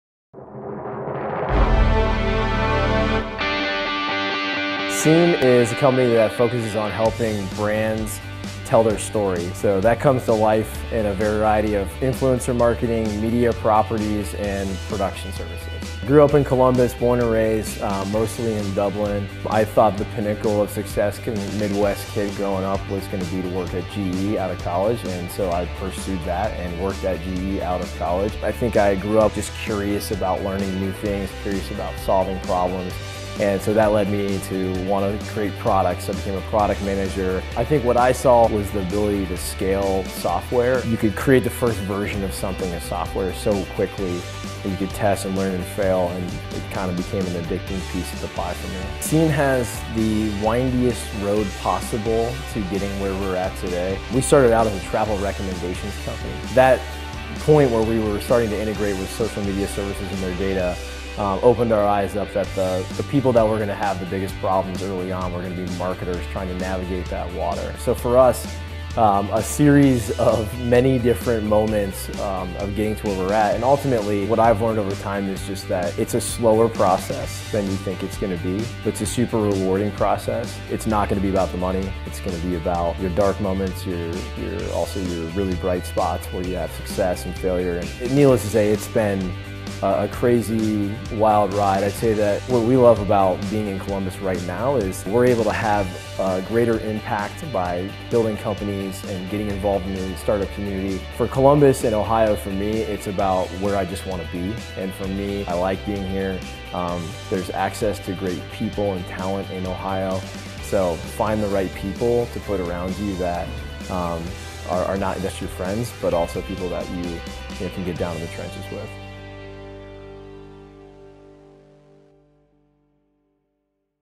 Founder Story: SEEN
SEEN in Columbus, Ohio, leverages the power of digital influencers to help move brand messaging forward in today’s social media-driven market. By matching top companies with individually chosen content creators, SEEN helps brands such as Hilton, AT&T and Reebok build trust, loyalty, and community naturally without appearing disingenuous or salesy. The company created its technology with support from Rev1 Ventures and the Ohio Tech Angel Fund, both central Ohio regional partners of Ohio Third Frontier.